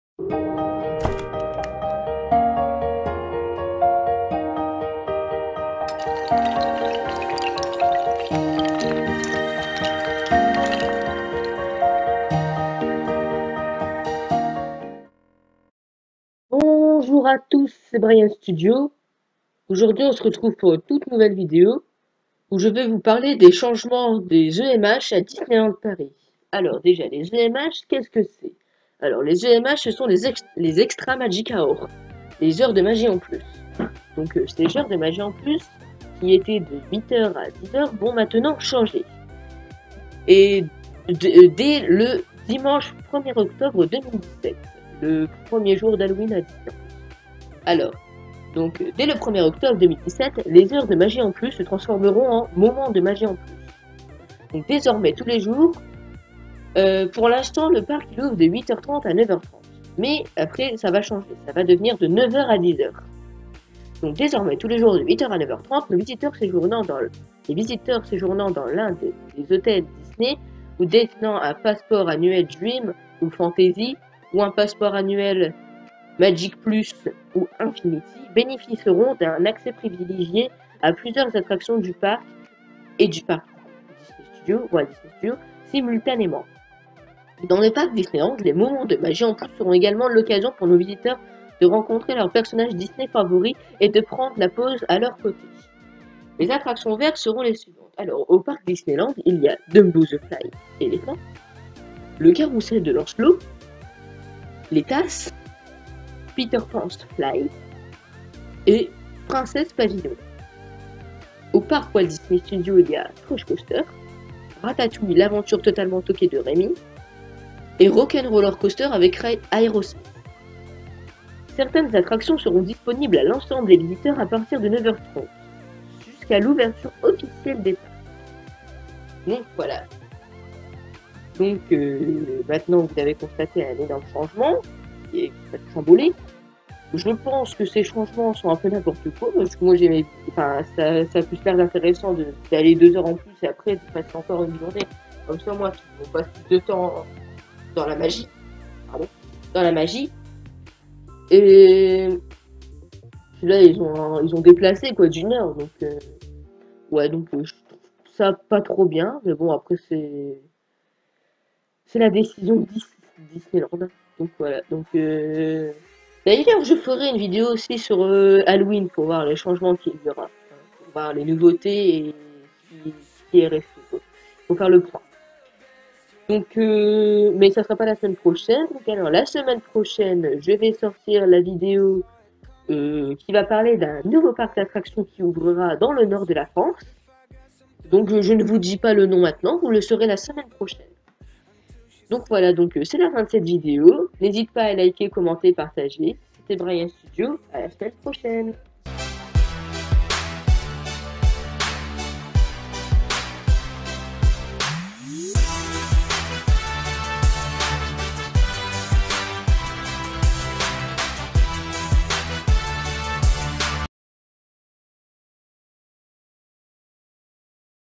Les changement des EMH à Disneyland Paris
Aujourd'hui je vais vous parlez des changement des EMH à Disneyland Paris alors le EMH se sont les Extra magic hour qui veut dire les heure de magie en plus et des le 1er octobre 2017 se la va devenir les moment de magie en plus et tout vas changer .